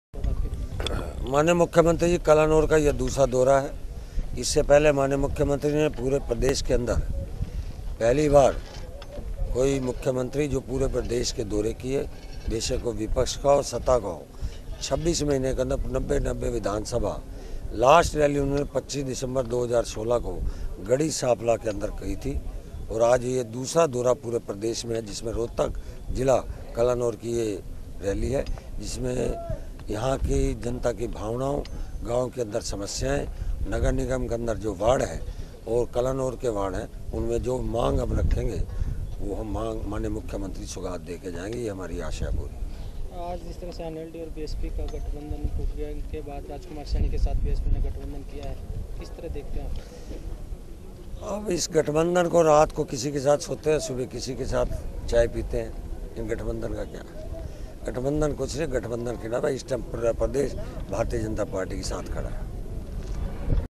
मनीष ग्रोवर के बिगड़े बोल रात को किसी के साथ सोते हैं सुबह किसी और के साथ चाय पीते हैं
रोहतक: सहकारिता राज्य मंत्री मनीष ग्रोवर ने बीएसपी और लोसुपा के गठबंधन पर तंज कसा है. उन्होंने कहा कि ये रात को किसी और के साथ सोते हैं और दिन में चाय किसी ओर के साथ पीते हैं.